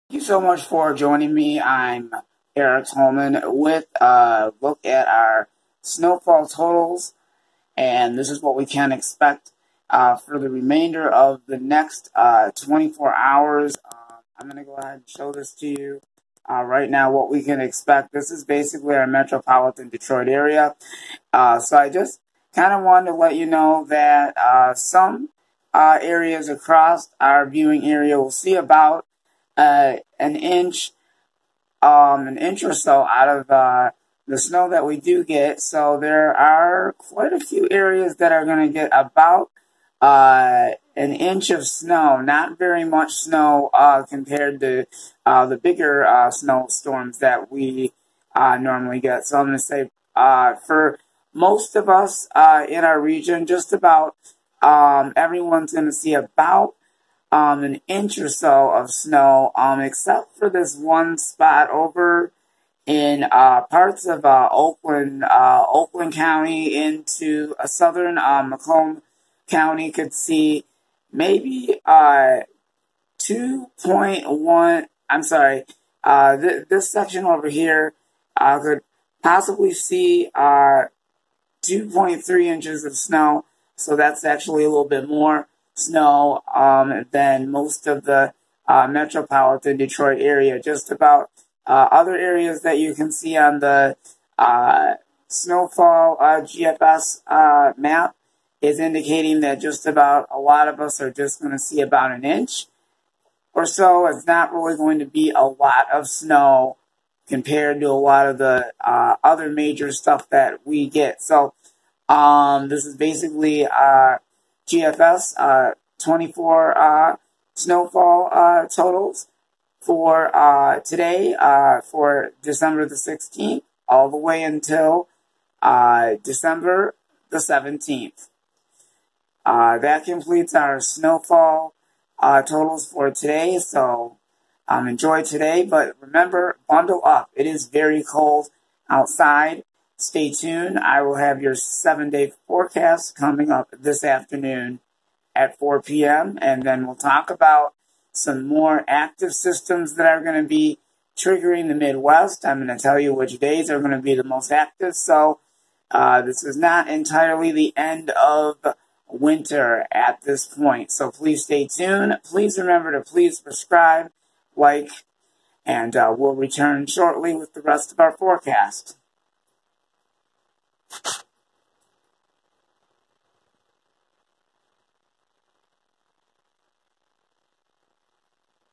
Snowfall Totals for Dec 16 thru Dec 17, 2020 SE Michigan
Snowfall accumulation totals across Southeast Michigan metropolitan Detroit viewing area.
everyone stay warm safe and bundle up it is very cold out today
will be right back after this